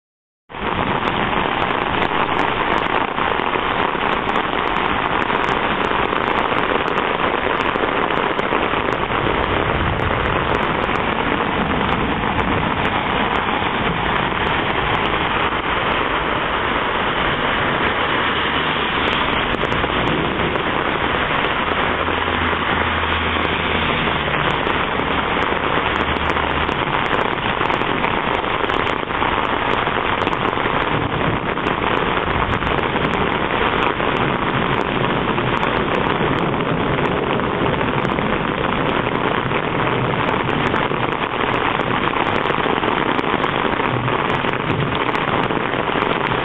Ice Storm NYC
Surprise ic storm hit New York City. Ice pieces covering the whole street and damaging cars and trees. Park Slope, Oct 11, 2010